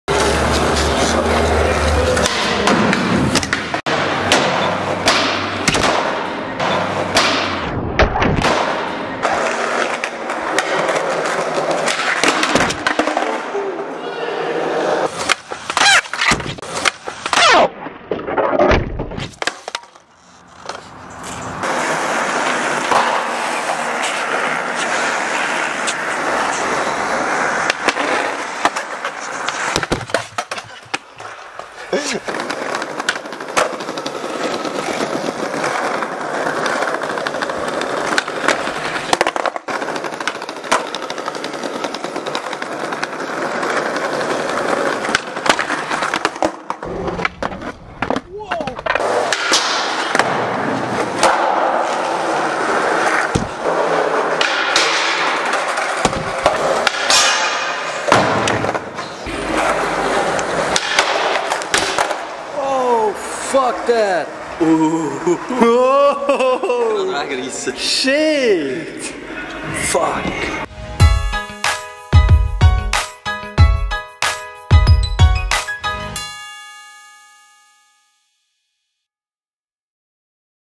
WORST Camera FALLS and BAILS Compilation
Here's my first WORST camera FALLS and BAILS Compilation and my Canon 60d is still alive. I'm gonna buy a new one soon...